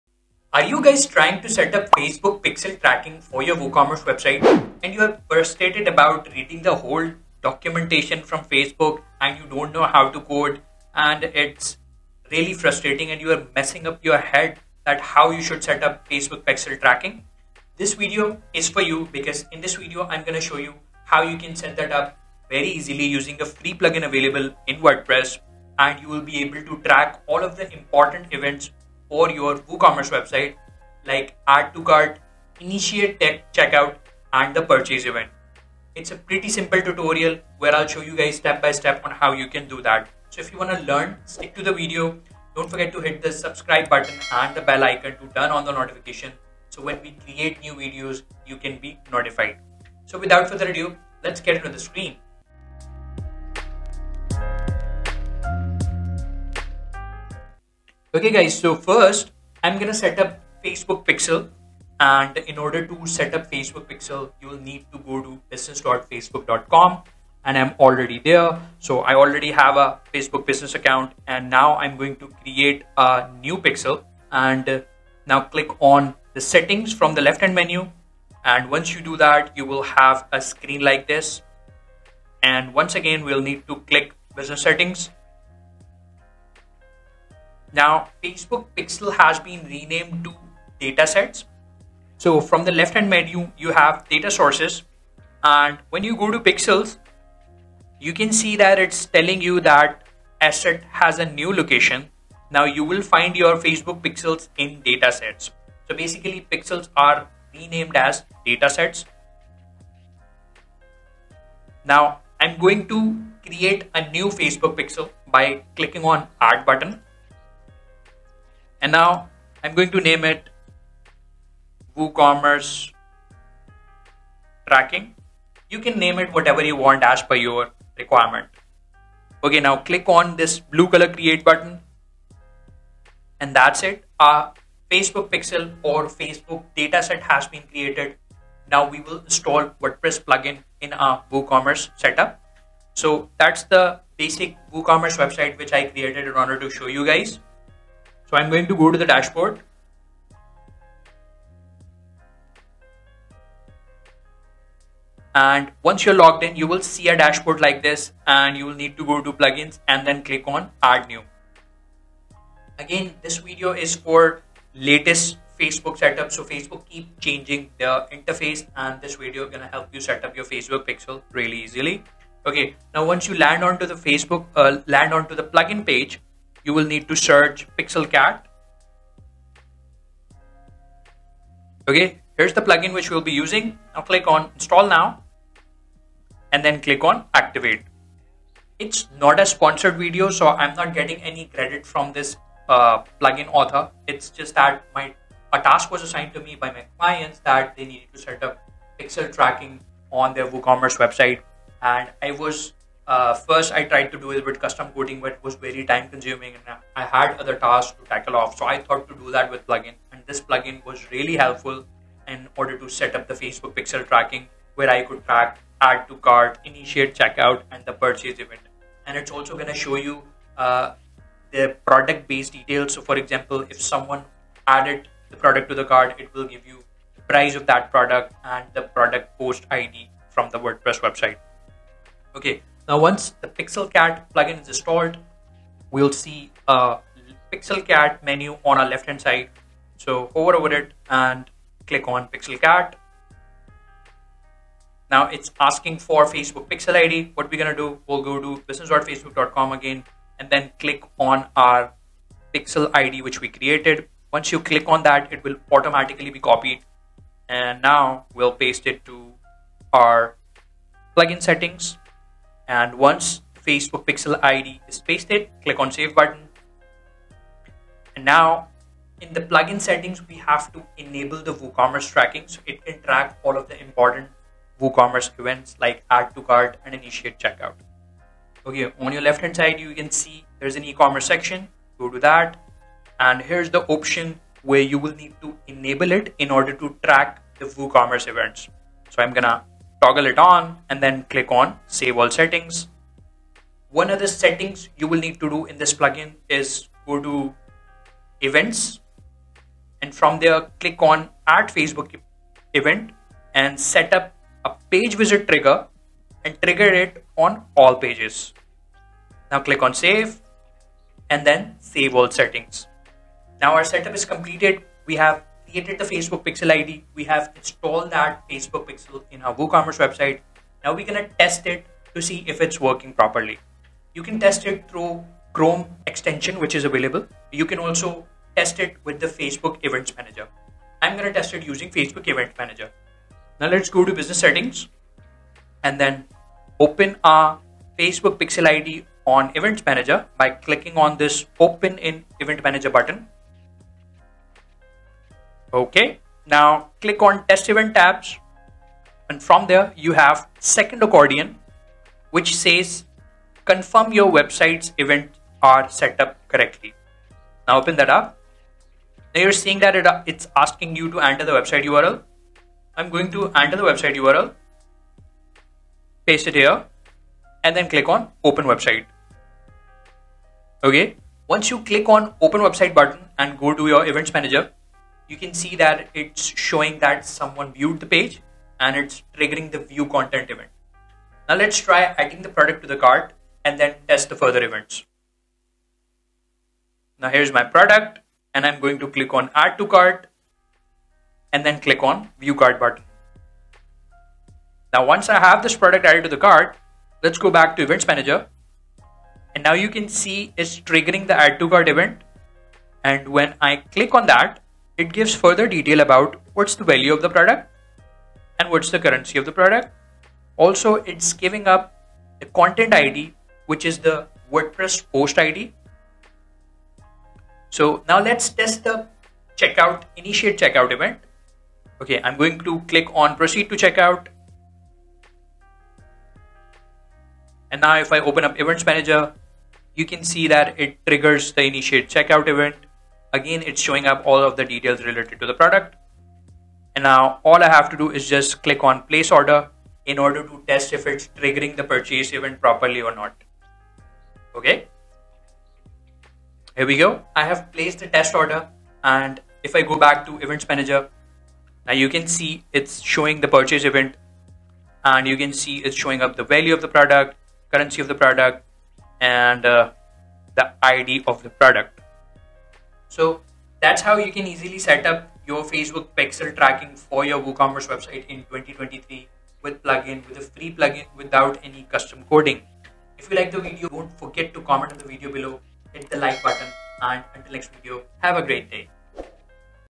How To Install a Facebook Pixel on WordPress - WooCommerce [Fast & Easy] In 2026
Facebook Pixel on WordPress WooCommerce

Are you struggling to set up Facebook pixel tracking for your WooCommerce website without coding knowledge?

Frustrated with lengthy Facebook documentation? In this step-by-step video tutorial, we've got you covered, Facebook pixel tracking using a free WordPress plugin, enabling you to track essential events on your WooCommerce site: add to cart, initiate checkout, and the purchase event

[Timestamps]
00:00 - Introduction
01:15 - Creating a New Facebook Pixel
02:55 - Installing the WordPress Plugin
04:27 - Configuring the PixelCat Plugin
06:26 - Testing the Facebook Pixel
07:30 - Conclusion